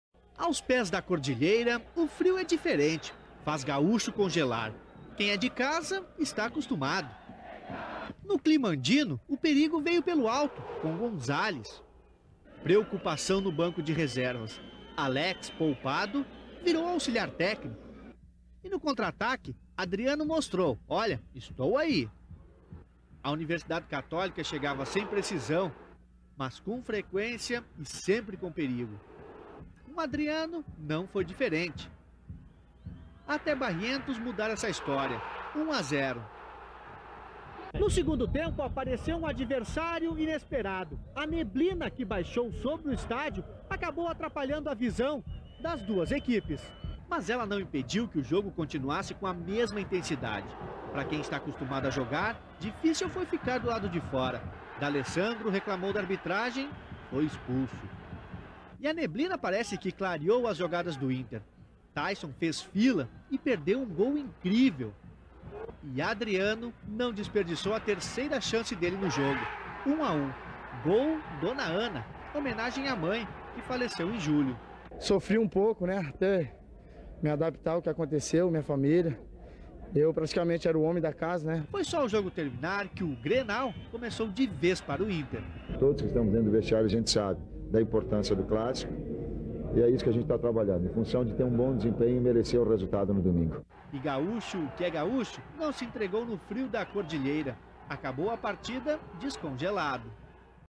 25/09/2008 - Universidad Catolica 1x1 Internacional - Copa Sul-Americana 2008
25/09/2008

Universidad Catolica 1x1 Internacional - Copa Sul-Americana 2008

Gol do Inter: Adriano